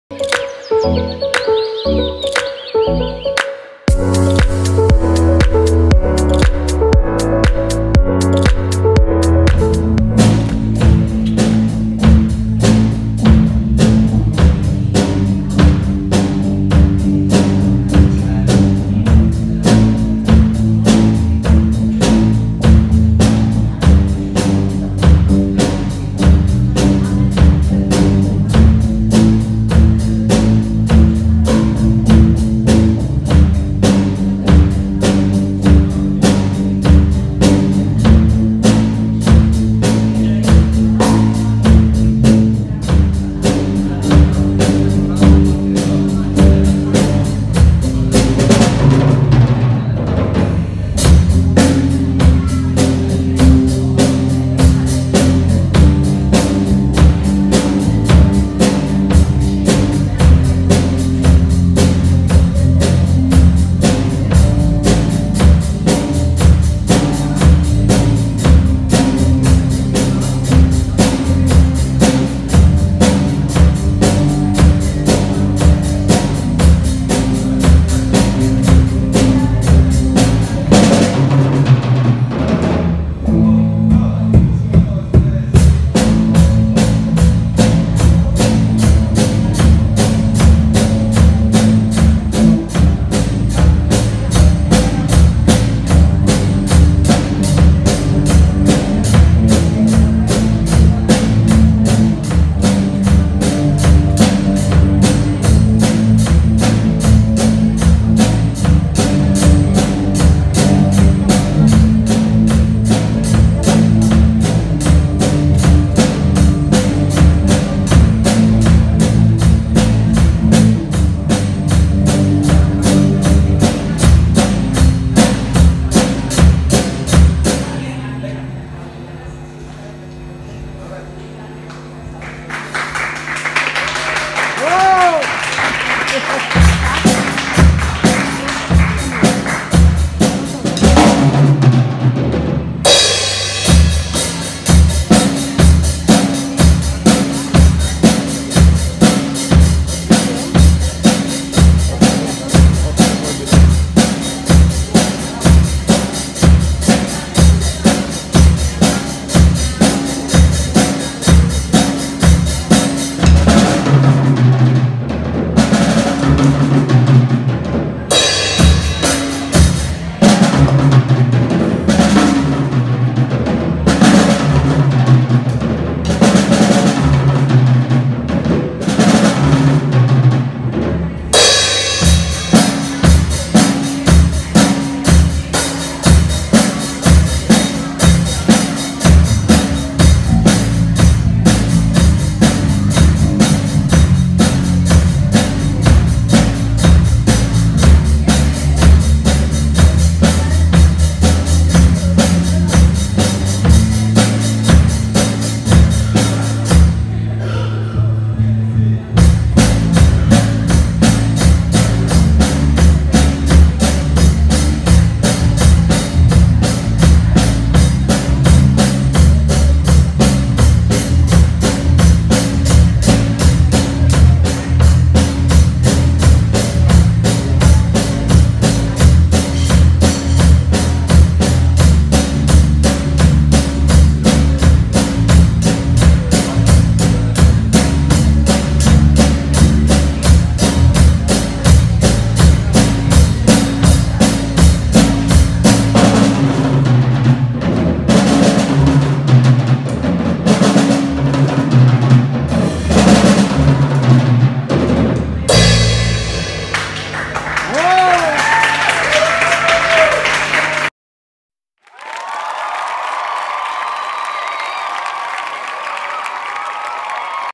The future best drummer - Juan “El Bateria” - Conciertos en Jendrix Estudios
Juan le encanta la batería y la guitarra eléctrica u está en sus momentos iniciales de su aprendizaje y lo hace bastante bien como lo demuestra en este concierto del 27 de Junio. A disfrutar de la música.